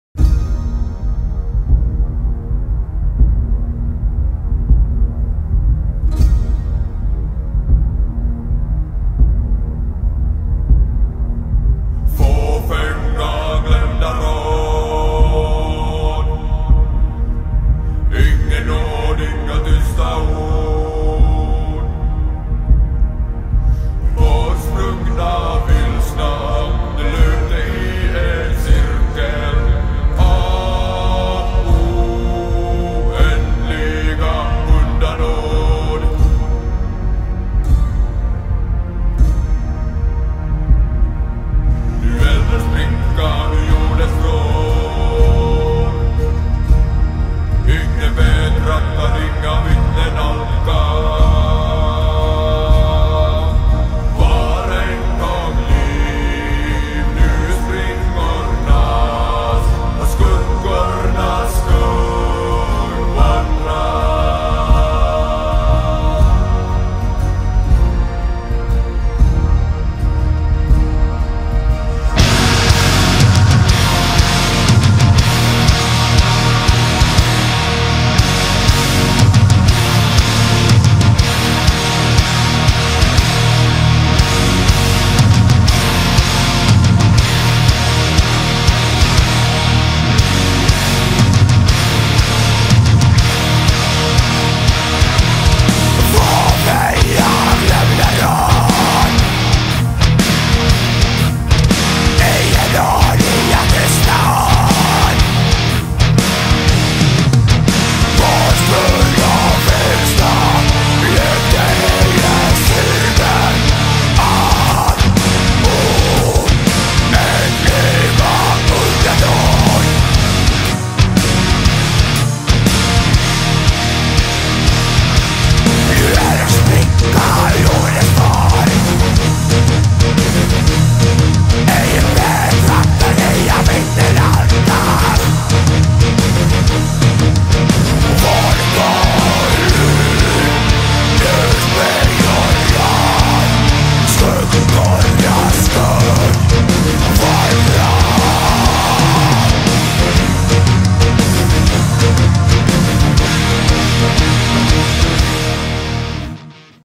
Boredom and a digital translator-Tristess och en digital översättare
This is a video explaining why i have my facebook statuses in swedish. I don't speak swedish at all by the way, i just listen to Finntroll.